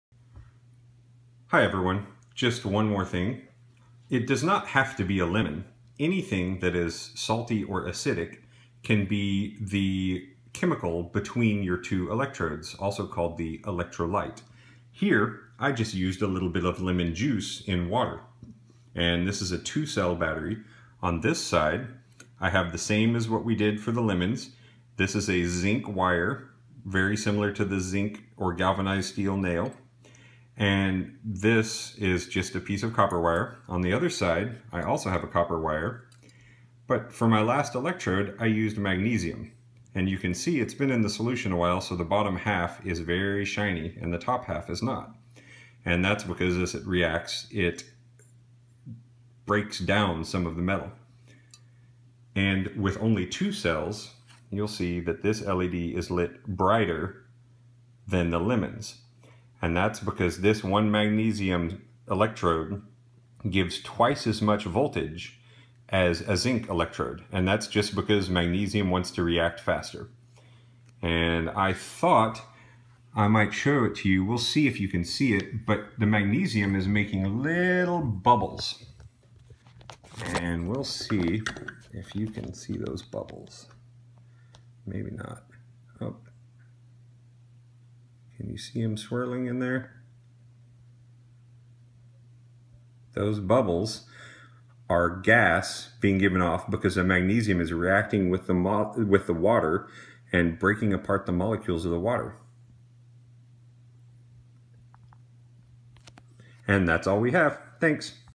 JD Lemon Battery 3 of 3
Part 3 demonstrates the alternate construction of lemon batteries for young children.  Please be nice, as we're trying to keep this light and simple for kids.